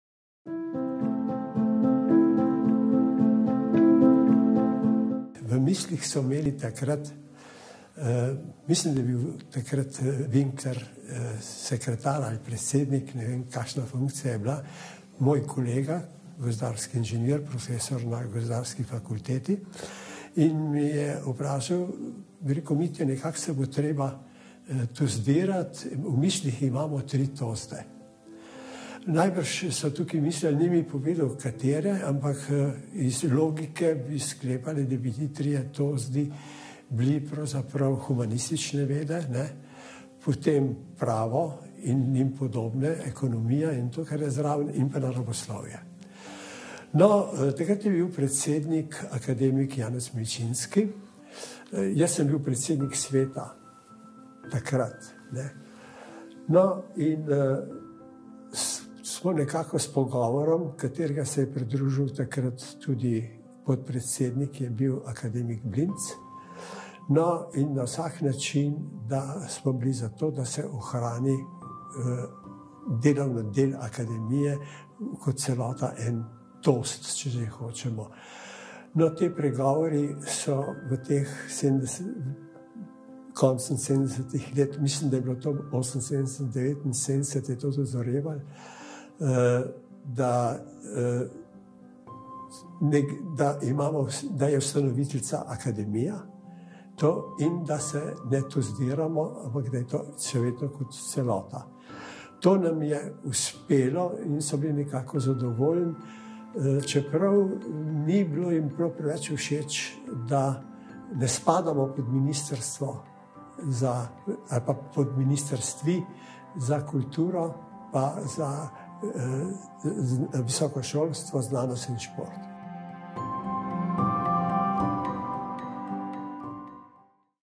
Izjava akad. dr. Mitje Zupančiča, prvega direktorja ZRC SAZU, o okoliščinah ustanovitve
Izjava akad. dr. Mitje Zupančiča, prvega direktorja ZRC SAZU, o okoliščinah ustanovitve javnoraziskovalnega zavoda
Mitja Zupančič, doktor gozdarskih znanosti, od leta 2001 redni član SAZU, med letoma 1982 in 1992 na čelu ZRC SAZU